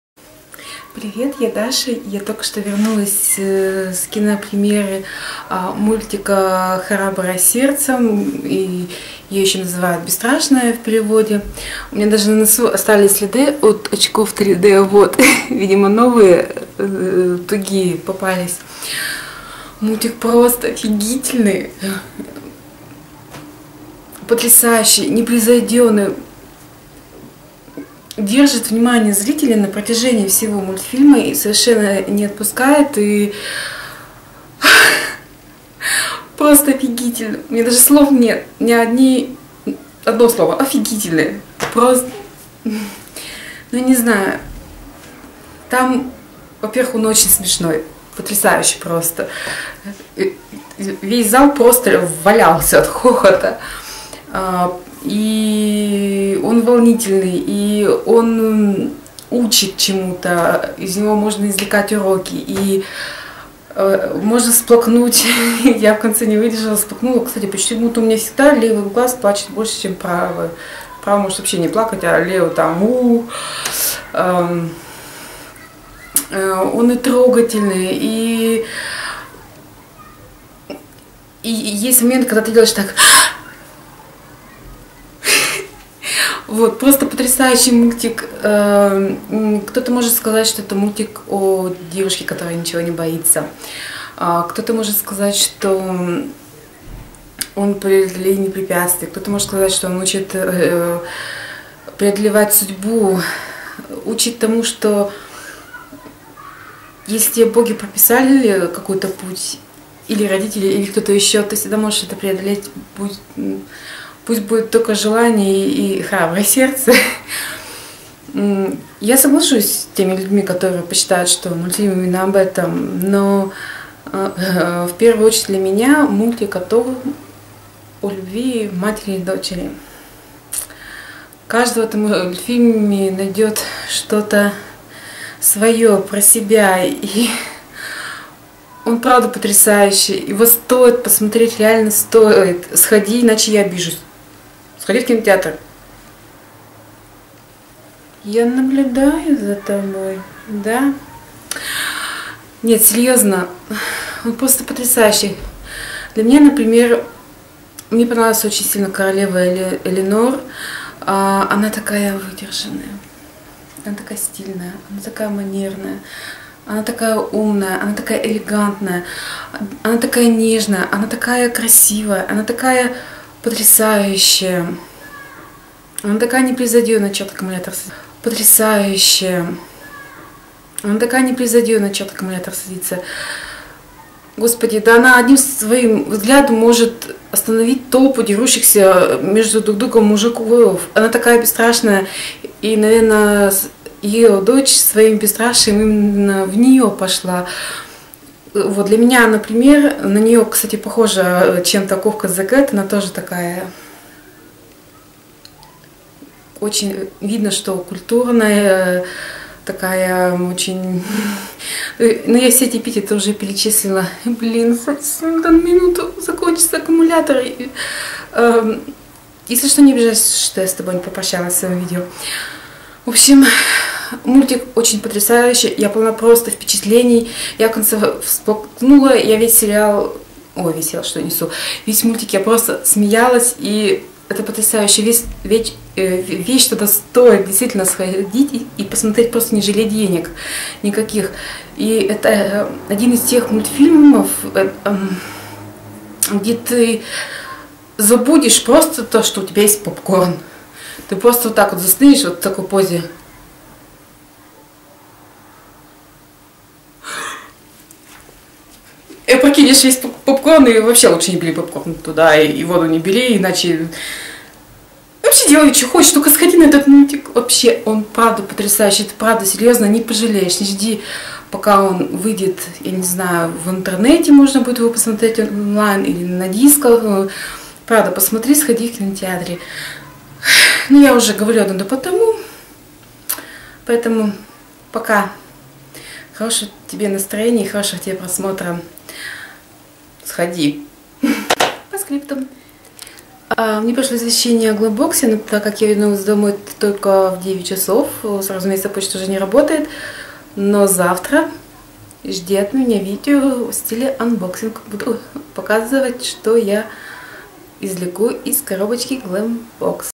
Влог: м/ф "Храбрая сердцем"
Мои впечатления от похода в кинотеатр